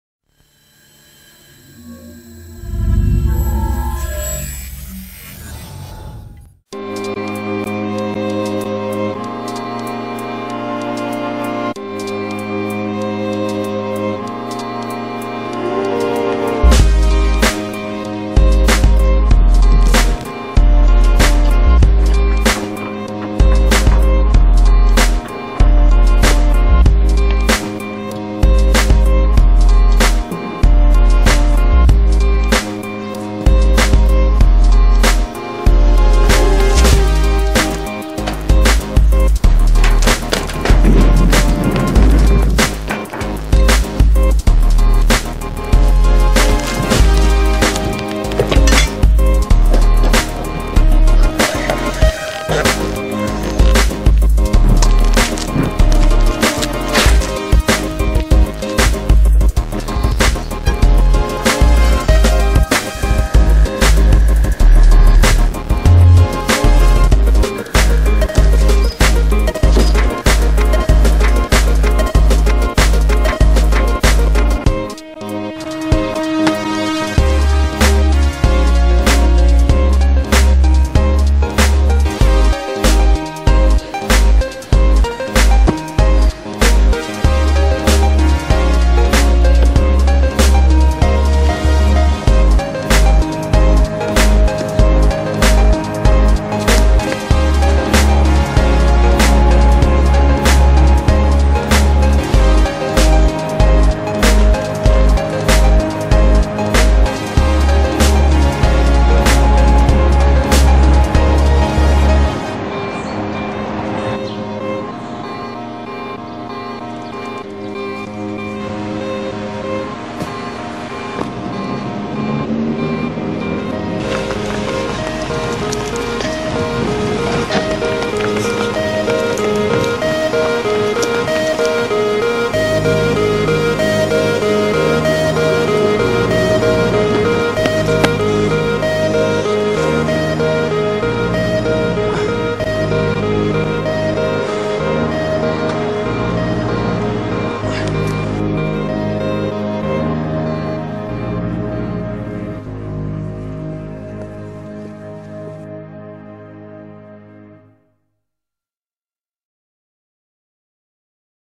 Freerunning Training .
just a few days here and there training, made to test new camera. Panasonic mini cam. Training in Kiev and Basingstoke. some of my Freerunning training and some creative angles.